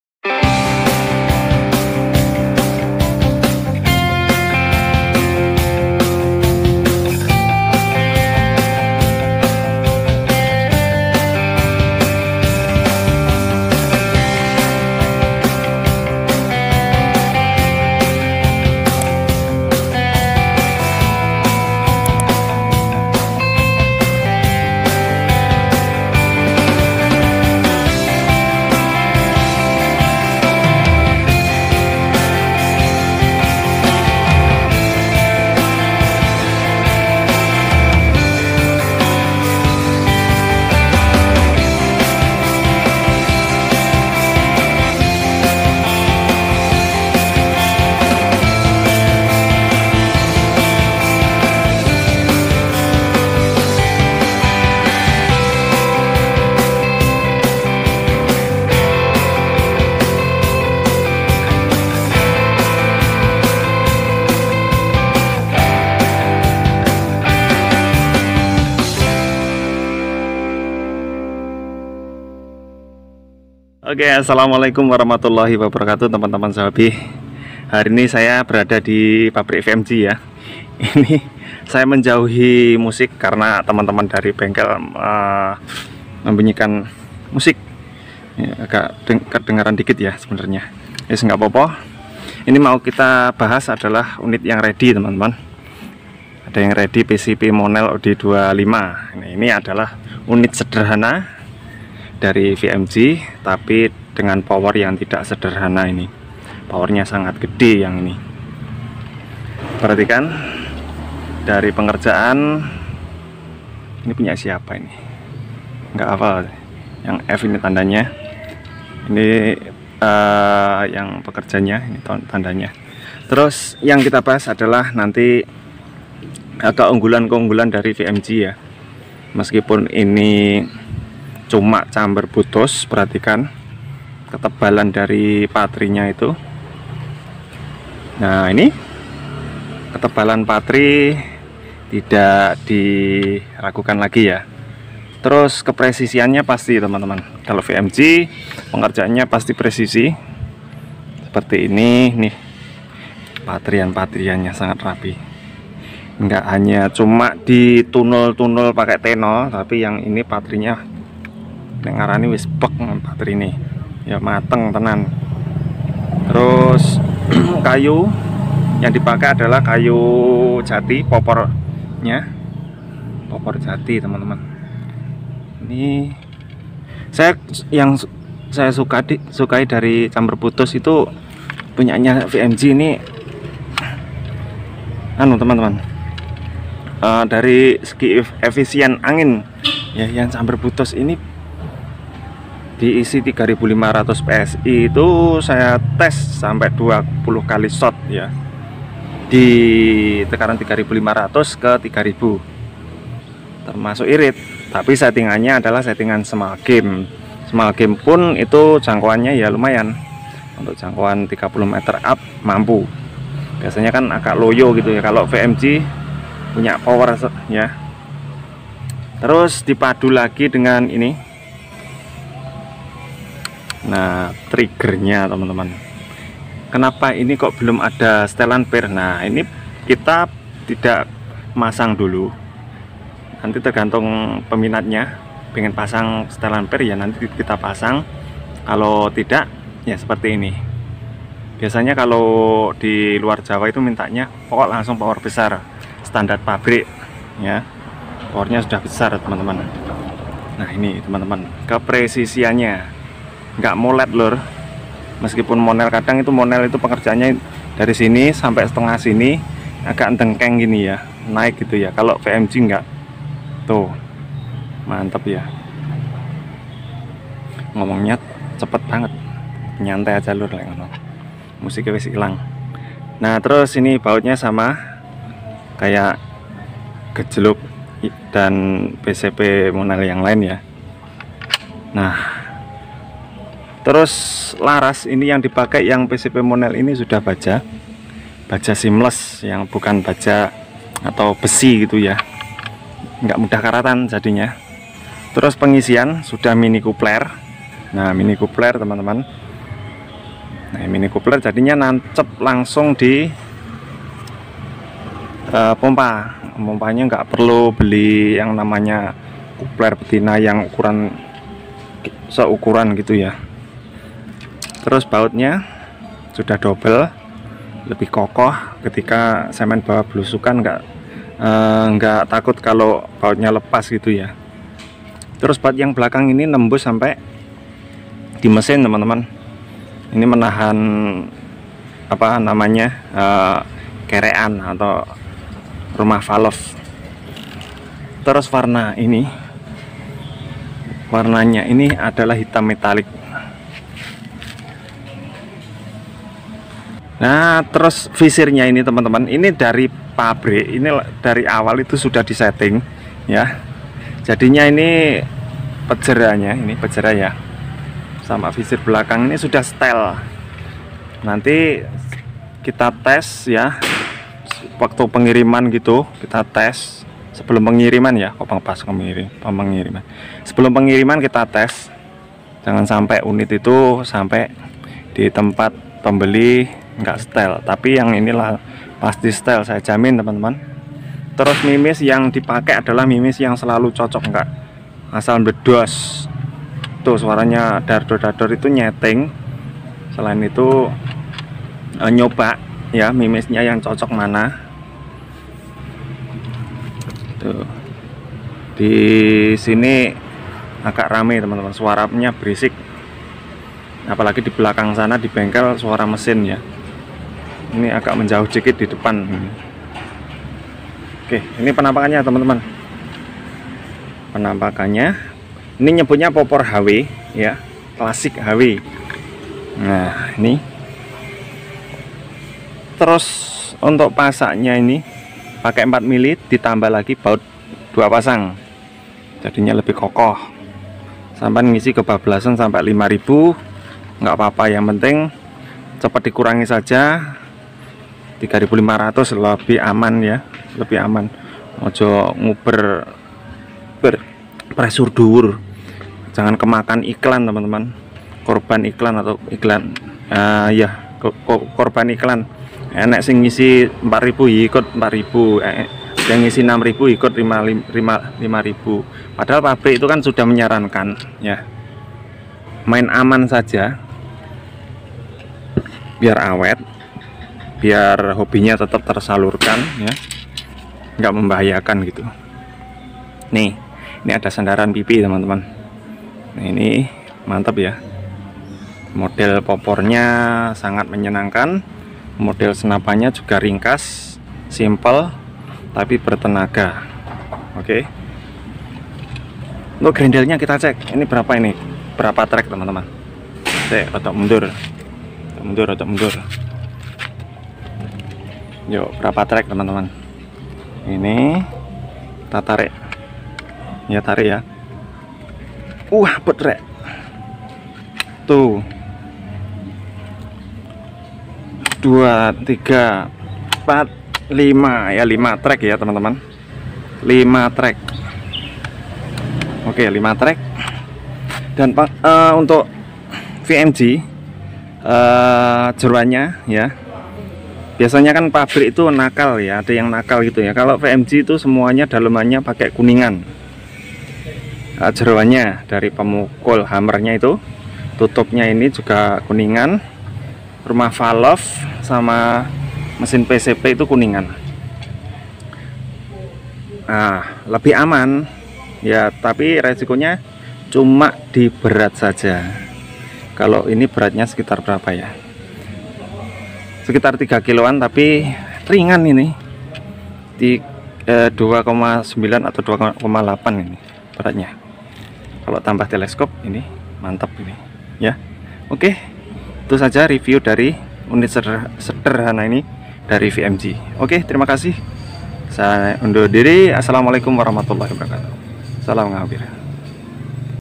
review langsung dari pabrik
yuk kita jalan jalan sebentar di vmg sambil review monel od25 yang satu ini